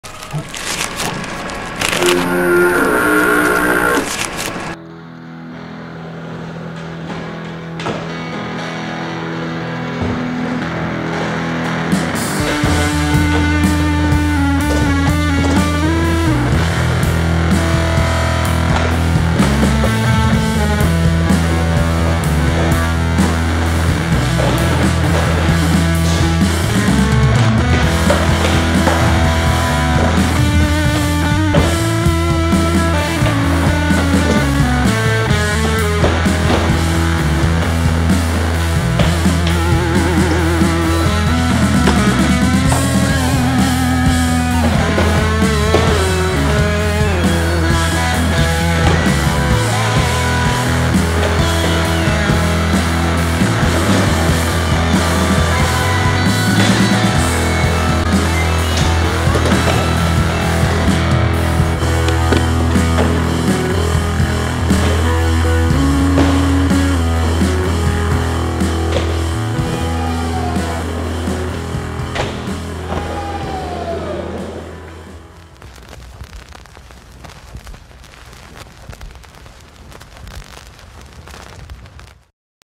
Park Barge: Riley and Tony Hawk
It was a rad thing to watch Tony and Riley Hawk skate our park together, do some doubles lines, and have Tony shoot some video of his own.